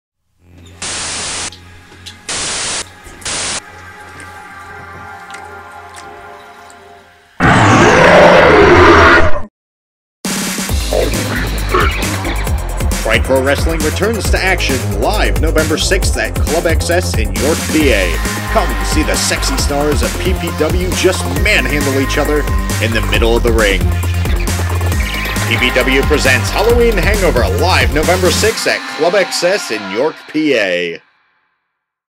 Pride Pro Wrestling Presents: Halloween Hangover
PPW is coming back to York, PA

Come see the hottest stars roll around in the ring to see who is the greatest LGBT grappler.